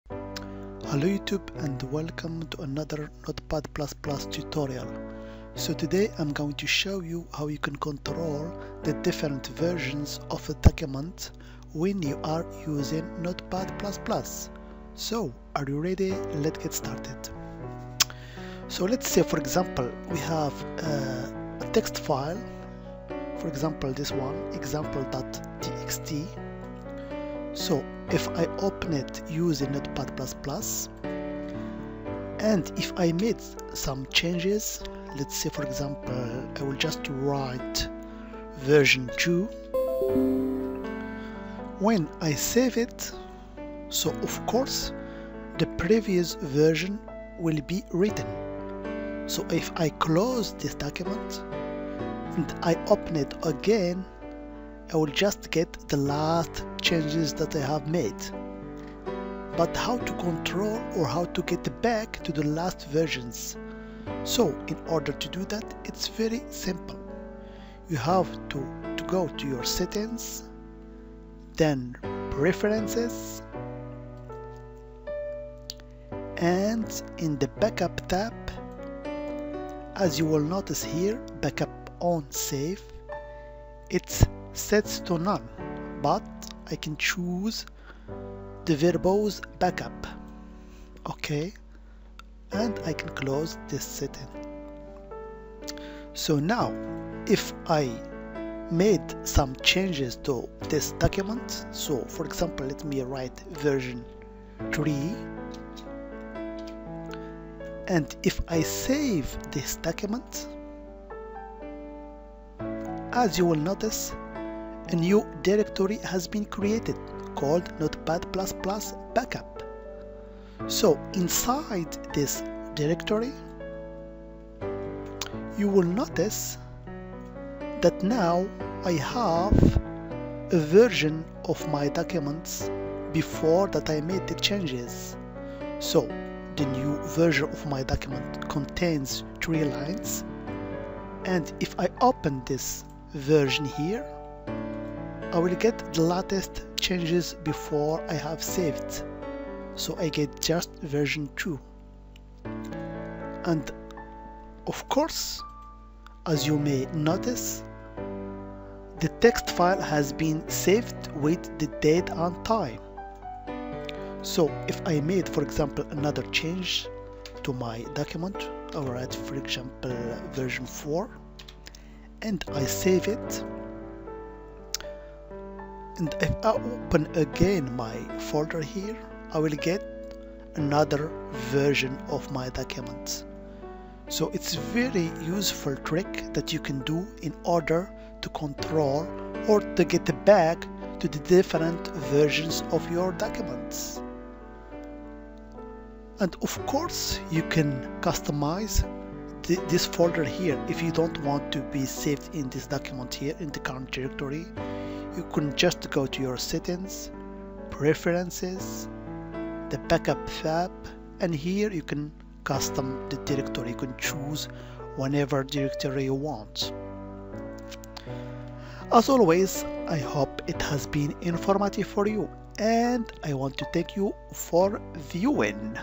Notepad++ Version Control Tips & Tricks
This video, I'll show you how you can use Notepad++ as a version control system, specially when you want to be able to revert back to a previous working version of your document.
To do that, you can use the Backup settings built into Notepad++ to take care of  Version Control. To set this up:
1- Install notepad++
2- Open Notepad++
3- Select Settings - Preferences
4- On the Backup tab, change the backup setting from None to Verbose.